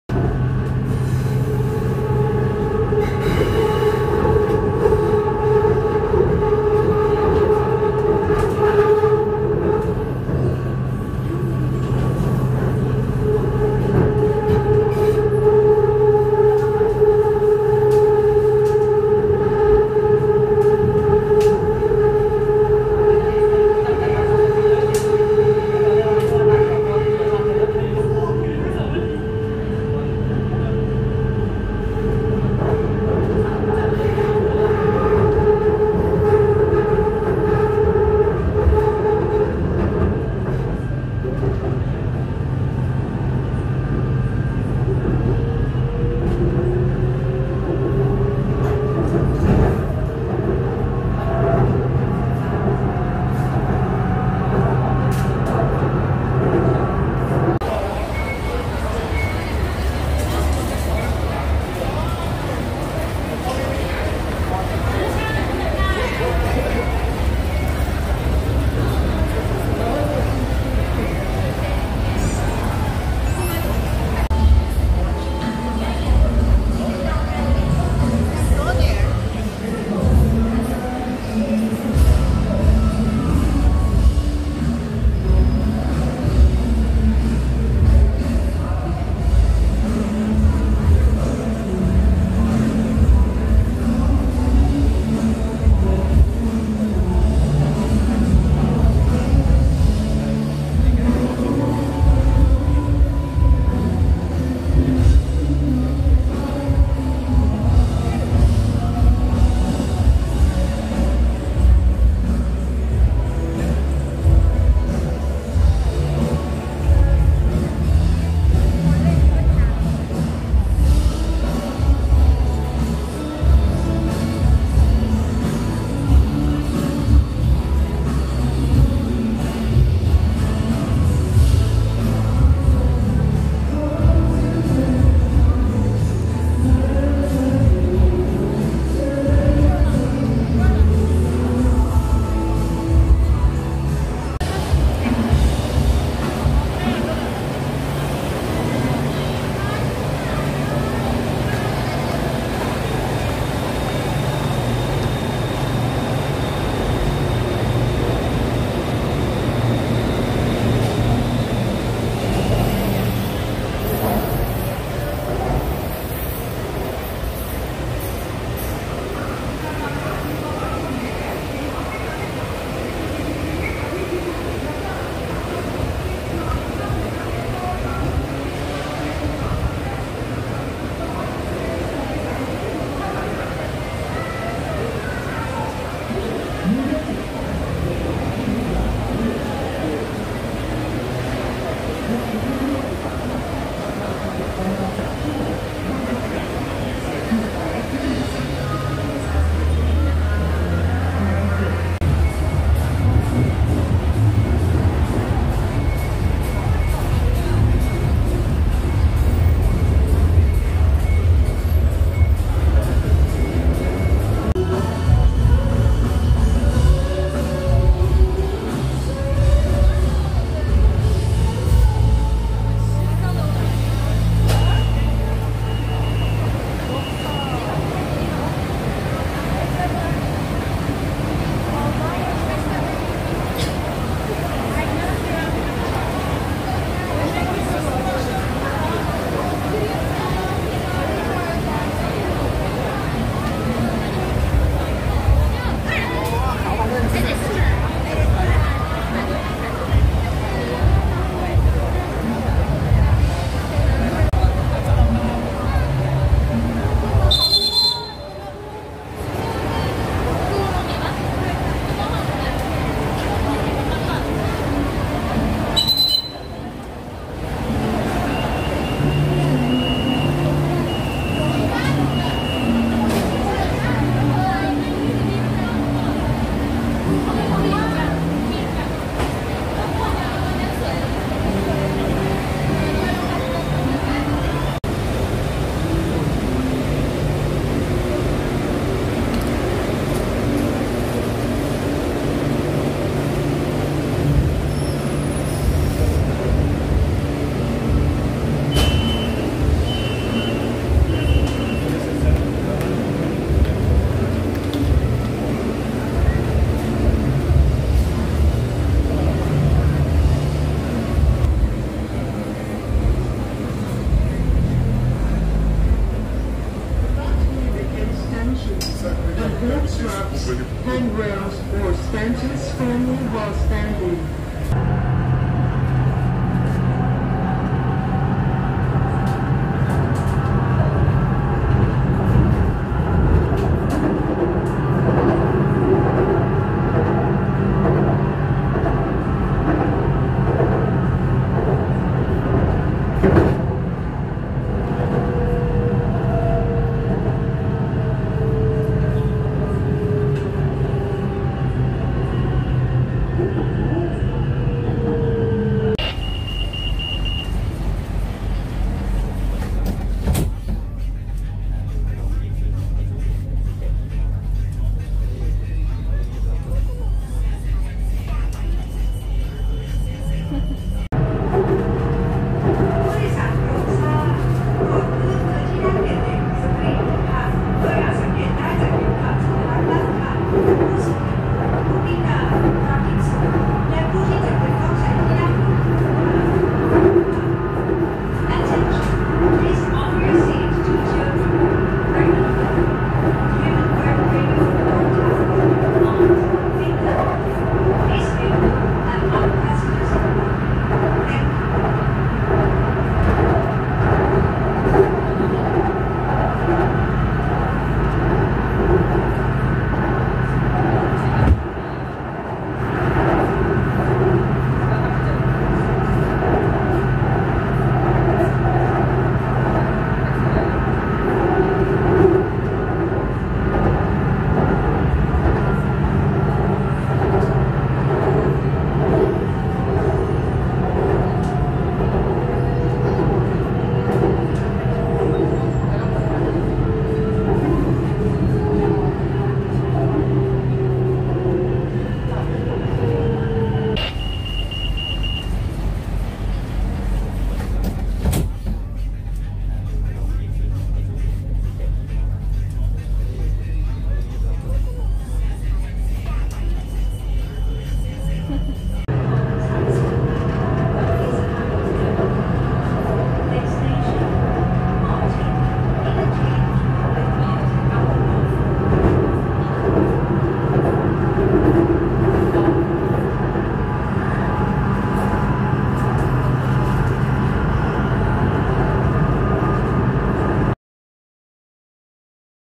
.Yaek Kor Por Aor skytrain station | Thailand tour 2024
Bangkok skytrain experience on Sunday 14th April 2024. Departure from Yaek Kor Por Aor BTS station. Have a good relaxing. Thank for joining me.
Yaek-Kor-Por-Aor_BTS_station_experience